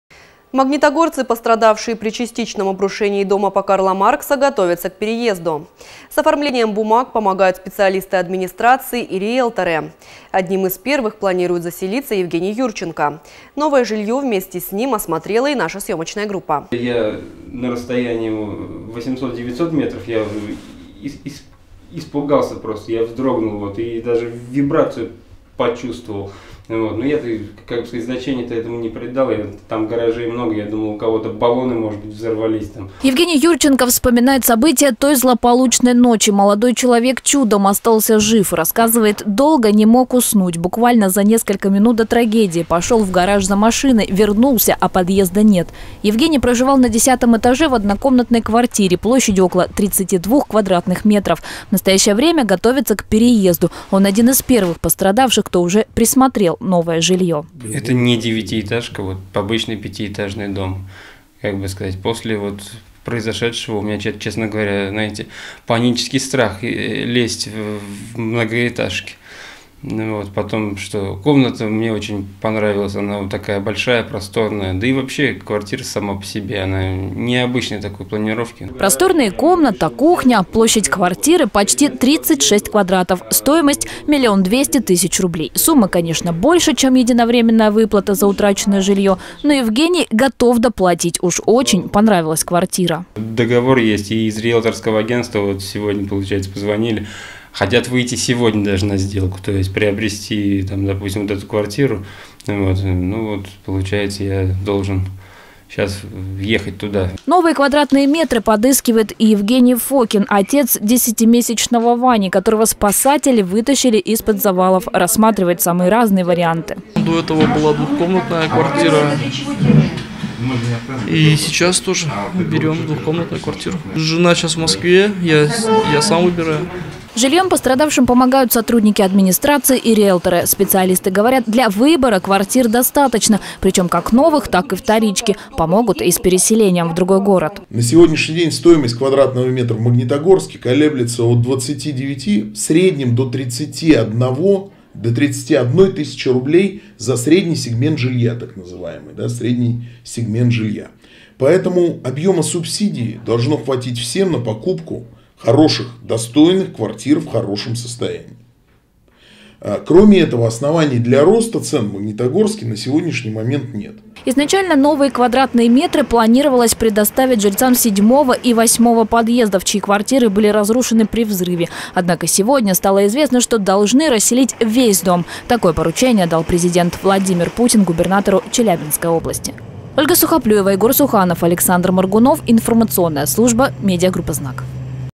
НОВЫЙ ДОМ
Магнитогорцы, пострадавшие при частичном обрушении дома по Карла Маркса, готовятся к переезду. С оформлением бумаг помогают специалисты администрации и риелторы. Одним из первых планирует заселиться Евгений Юрченко — молодой человек чудом уцелевший при взрыве.